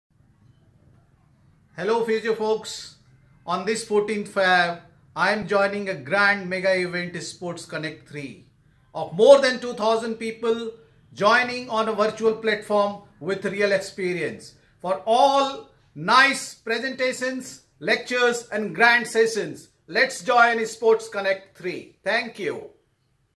Dr. Sanjiv . K Jha, Founder and Director of C.C.D.R. welcoming you all to attend SPORTS CONNECT 3
Dr. Sanjiv . K Jha, Founder and Director of C.C.D.R. welcoming you all to attend SPORTS CONNECT 3 on 14th February 2021